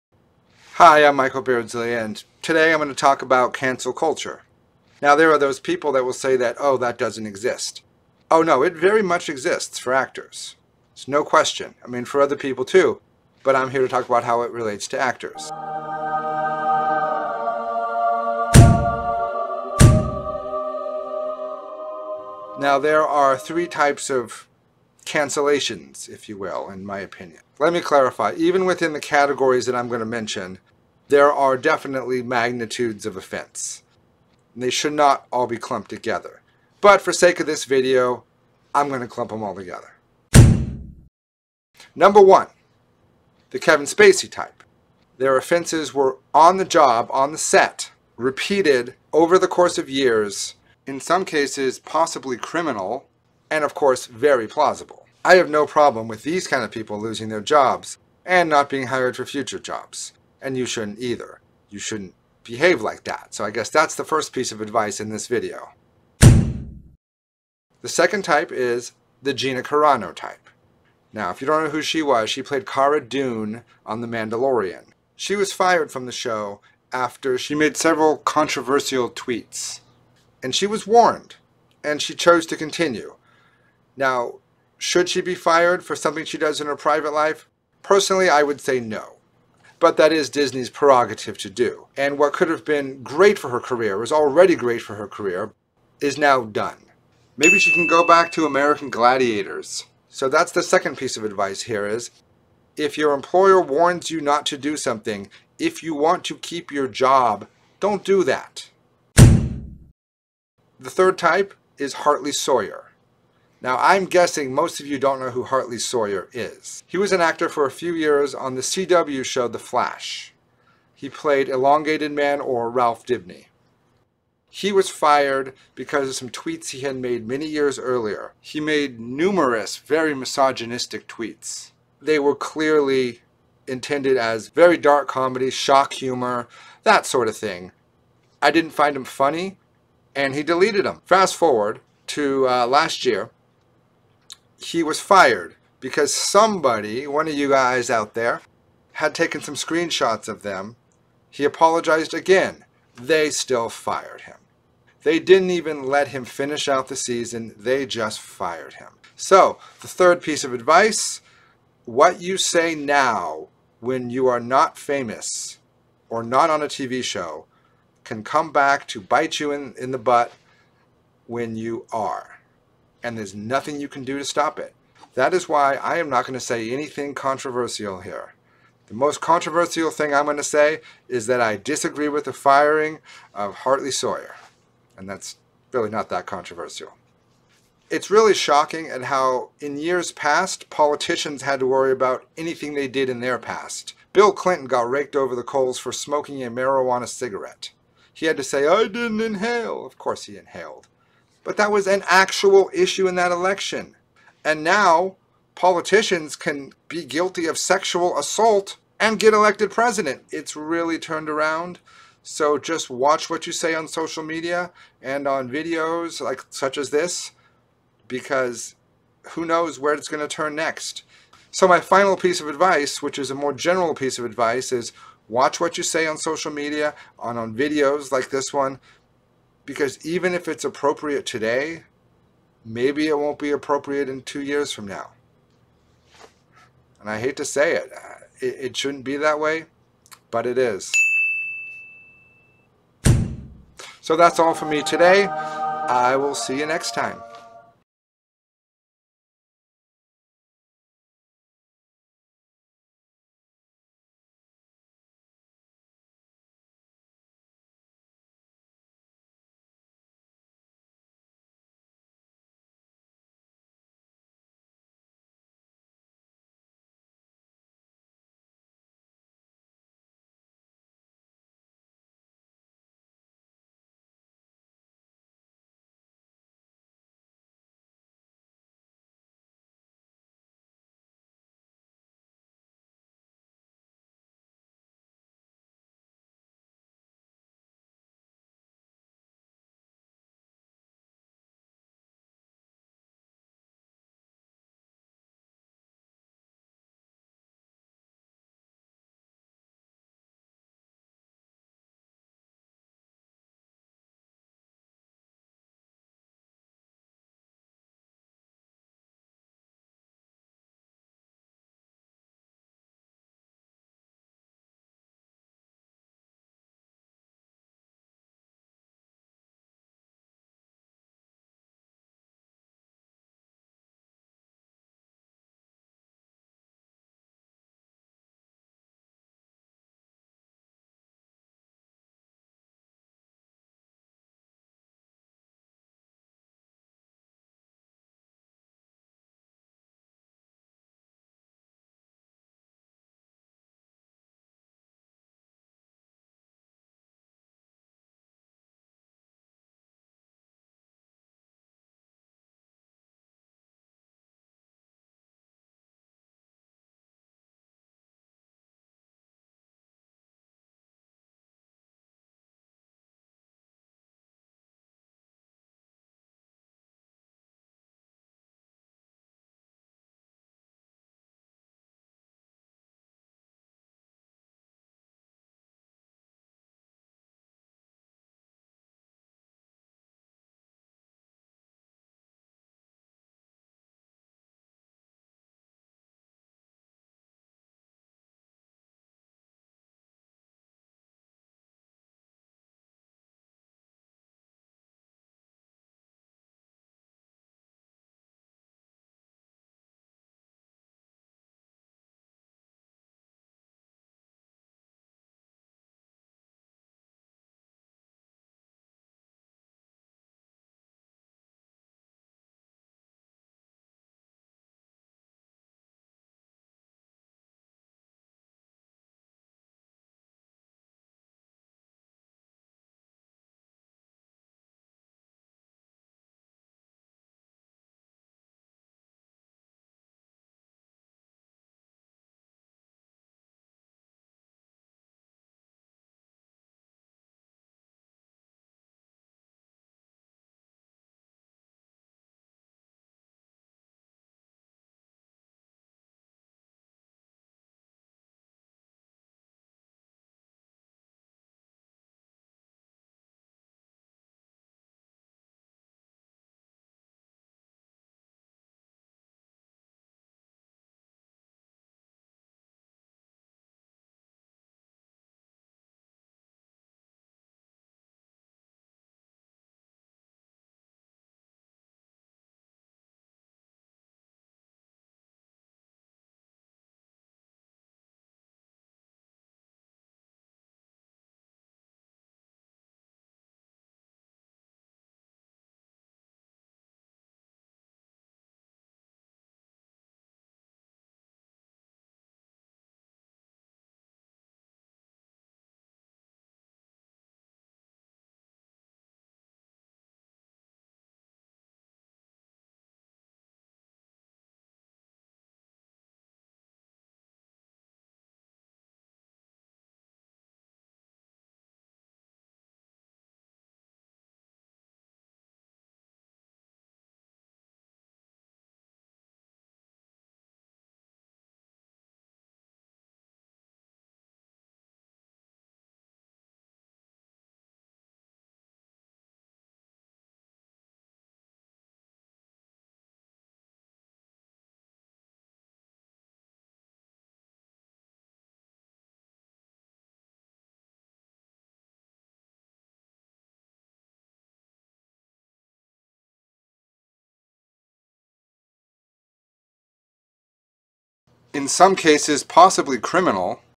Advice for Actors - Cancel Culture
It's my first "So You Want to Be an Actor" in years, and this is one that was not such a big deal back when I started doing these videos. Cancel Culture. It's not just in the movie industry, but that's my area of expertise, so that's what I'm gonna talk about.

Cancel Culture is real, sometimes it's 100% justifies, necessary even, other time maybe not so much. In this video I will go over three very different example of actors who have been canceled: Kevin Spacey, Gina Carano, and Hartley Sawyer.